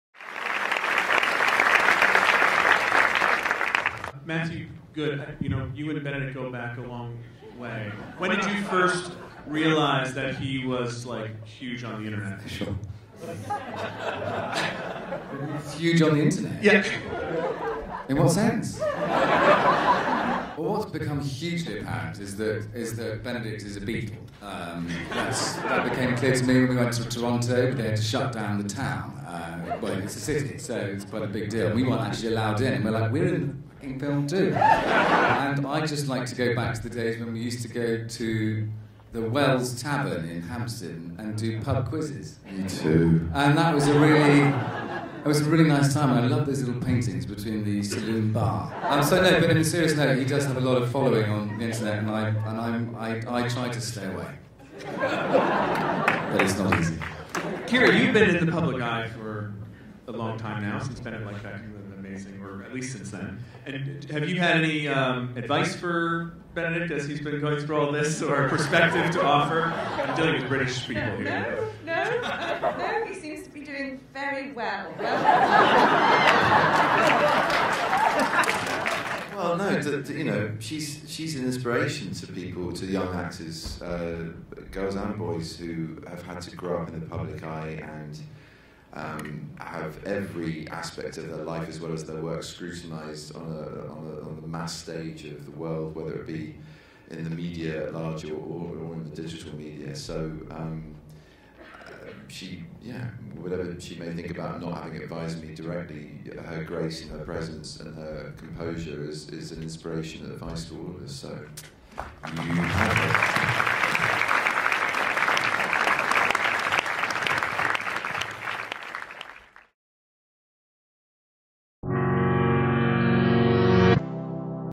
Benedict Cumberbatch's Co-Stars Tease Him About His Rabid Fans
During a Q&A discussion with the cast of The Imitation Game, Vanity Fair digital director Mike Hogan asked Matthew Goode about the fervor around Cumberbatch, his friend of 15 years.